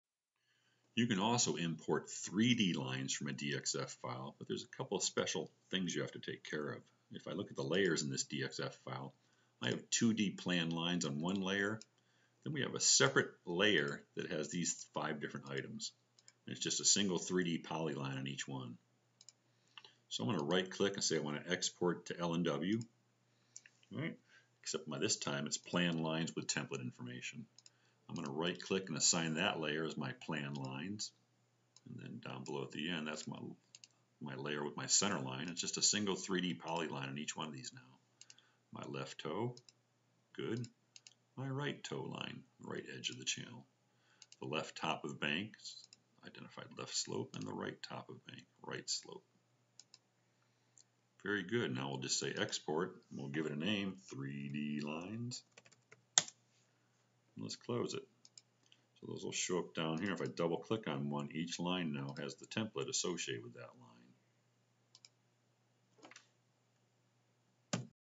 HYPACK - Shell: Export 3D DXF to LNW
HYPACK founder Pat Sanders demonstrates how to export 3D planned lines from a 3D DXF using the Export to LNW feature in the HYPACK Shell.